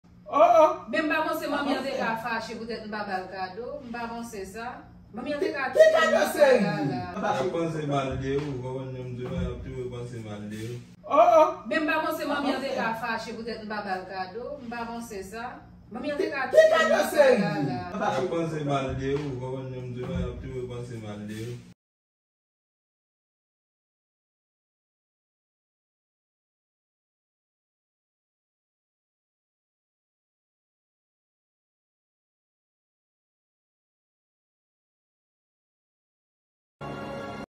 TI GOUTEMW BAY MOM KADO FÈT LA M PA BAY MOM JR LI FACHE PRANK 😂 PITA 7:00
Michelle Official MichelleOfficial prank.